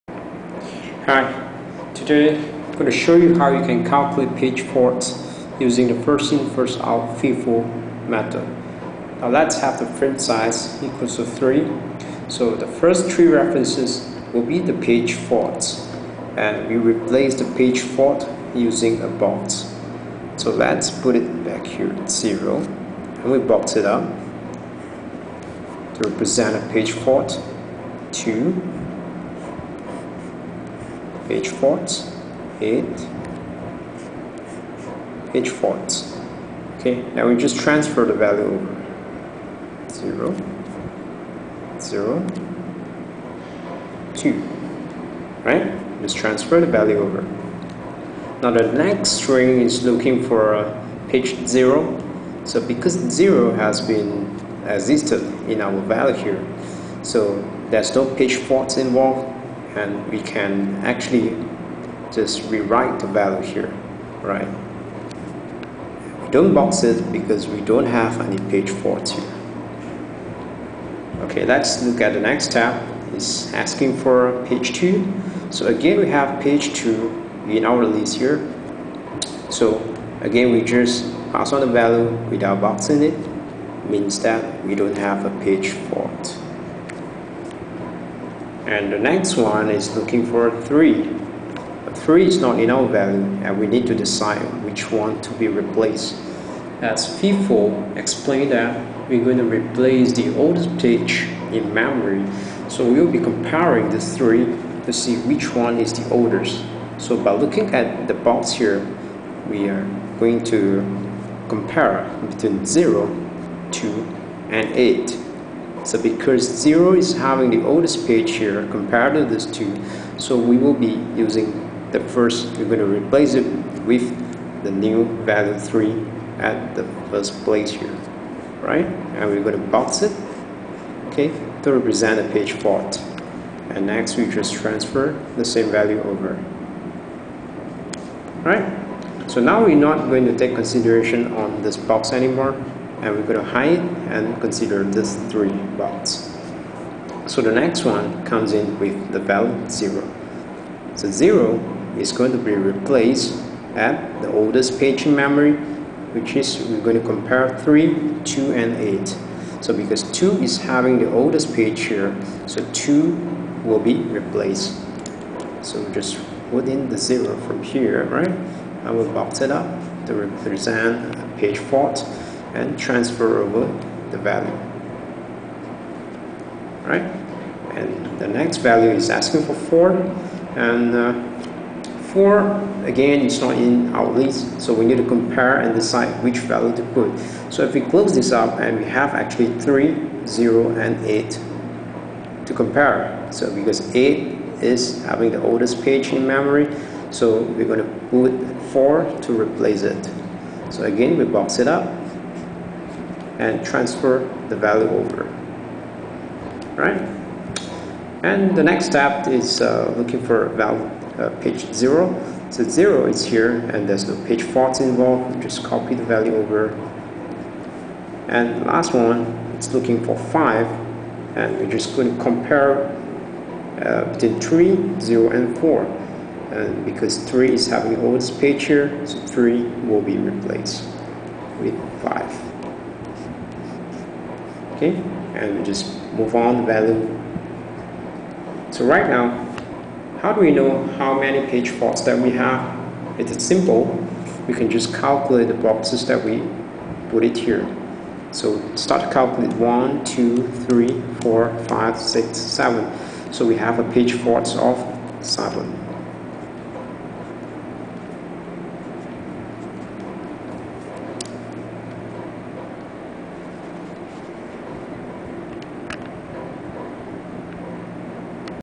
FIFO Algorithm
Calculate the page faults using FIFO with 3 as the frame size.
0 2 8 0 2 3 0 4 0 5
FIFO concept - Replace Oldest Page in memory